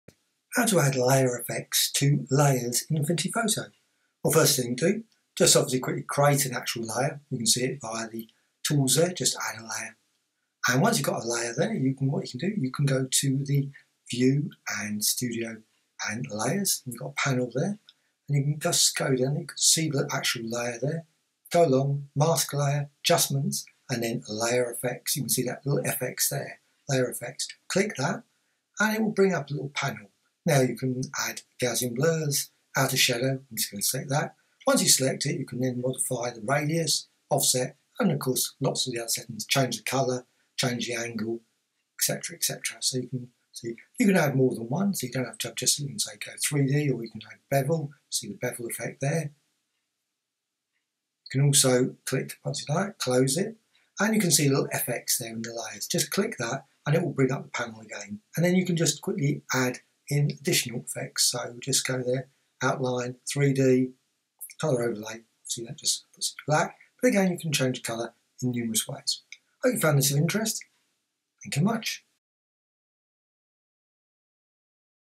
Add Layer Effects In Affinity Photo | How To Tutorial | Bevels 3D | Graphicxtras
How to add layer effects to a layer in Affinity Photo - layer effects can be used to add bevels, shadows to layers in Affinity Photo for all kinds of wonderful artwork